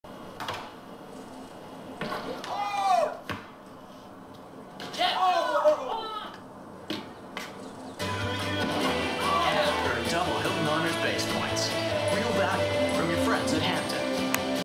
Third Hampton Inn Basketball Commercial | Legendary Shots
The commercial we were in.  Advertises the Hilton HHonors System.  Stay at a Hampton Inn! (Commercial Currently Airing) (Not really currently airing, we wrote that four years ago)

Add us on Snapchat! LegendaryShots

For some behind the scenes and exclusive content, go here!

Like our shirts? You can get them here!